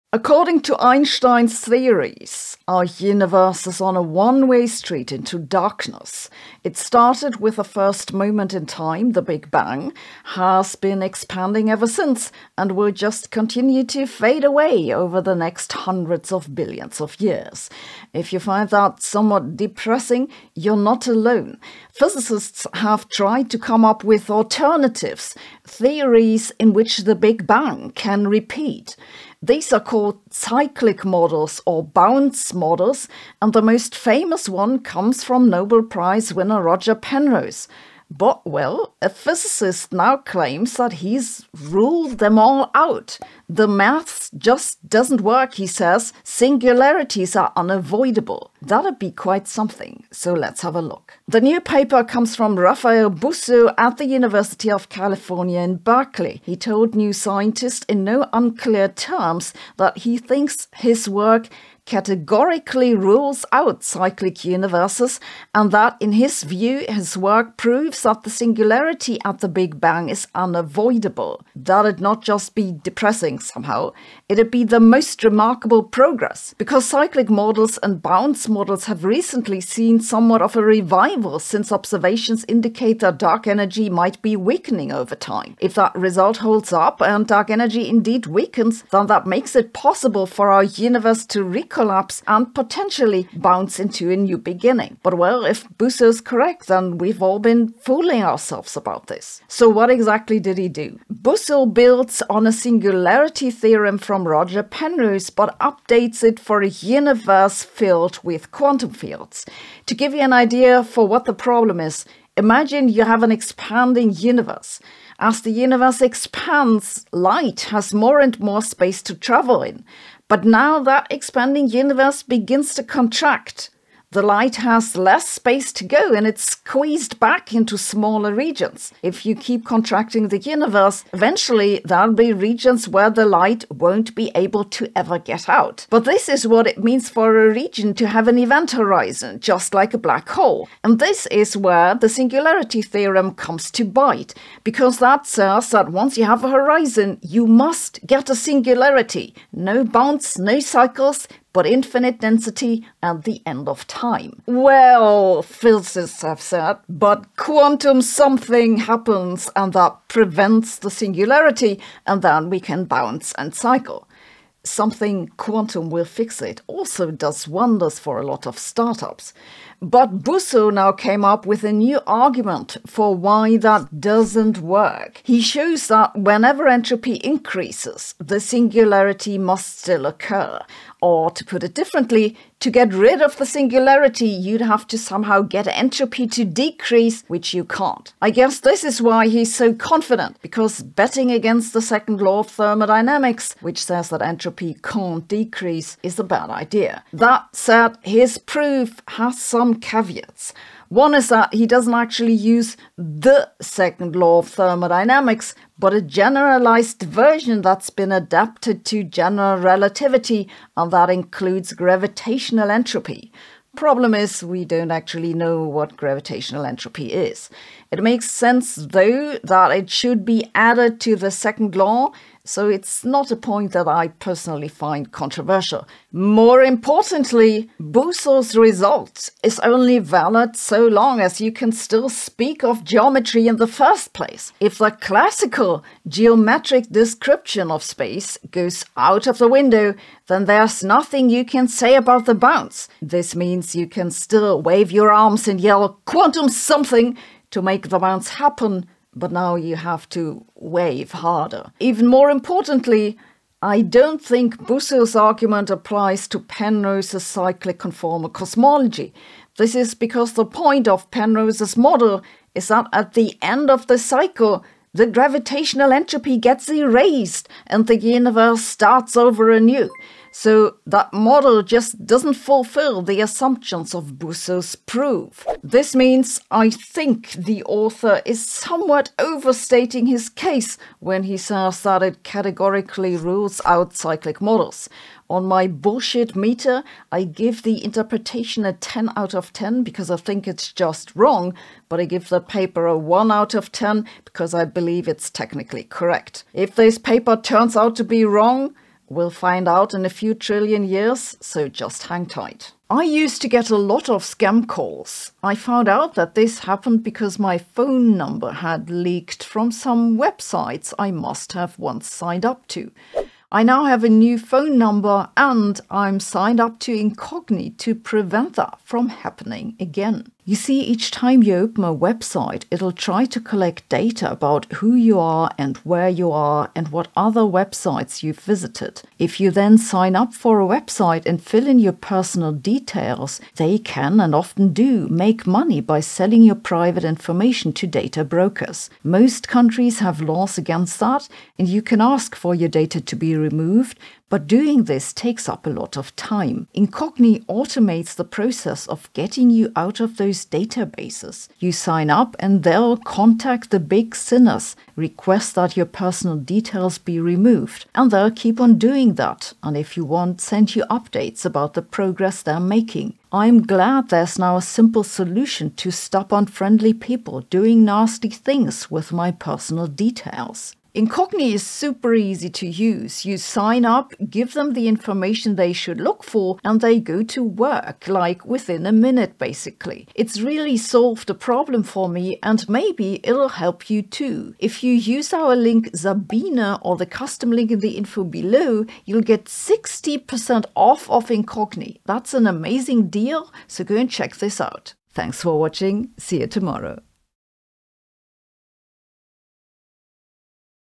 Big Bang Can't Repeat, Physicists Proves
According to Einstein’s theories, the universe started with a Big Bang singularity and is slowly expanding until it disperses into nothingness. But physicists have also come up with theories claiming that the Big Bang was non-singular and can repeat, restarting the cycle over again. These are called “cyclic models,” and they’ve re-emerged into the spotlight now that there’s mounting evidence that dark energy is weakening over time. However, a physicist from UC Berkeley recently published a paper which he claims “categorically rules out” cyclic models. Let’s take a look.